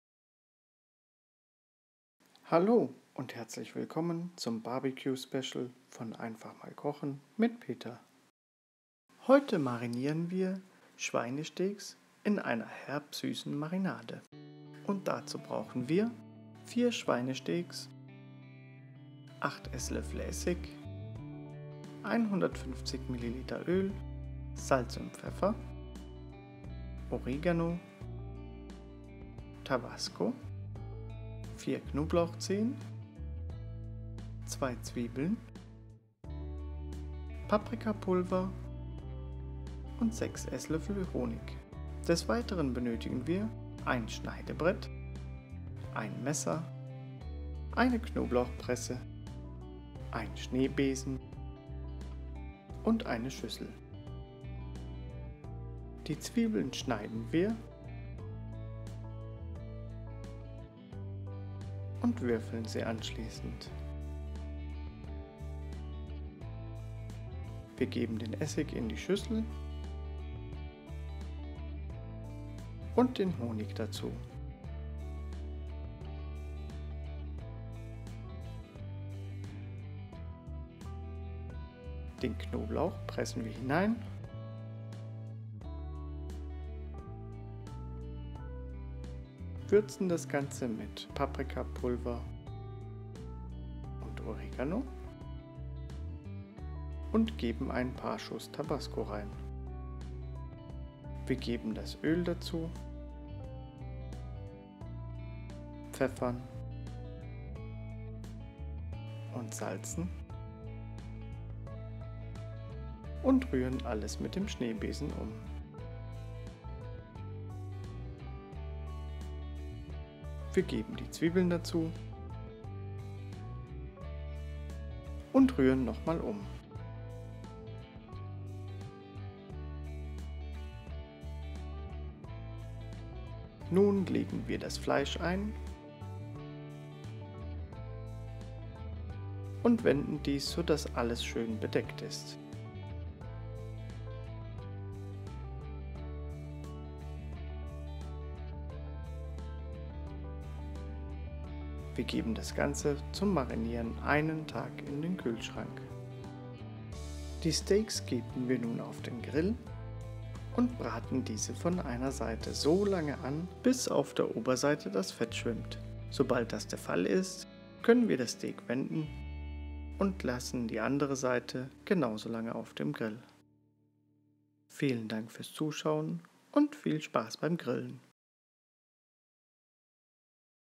BBQ Special 10 - Herb Süße Marinade
!!!Achtung 1 Tag Vorlauf benötigt!!!

Herrzlich Willkommen zum BBQ Special von "Einfach mal Kochen"

In diesem Video bereiten wir Steaks mit einer Herb Süßen Marinade vor.

Zutaten (4 Steaks):
4 Schweinesteaks
8 EL Essig
150 Milliliter Öl
Salz und Pfeffer
Oregano
Tabasco
4 Knoblauchzehen
2 Zwiebeln
Paprikapulver
6 EL Honig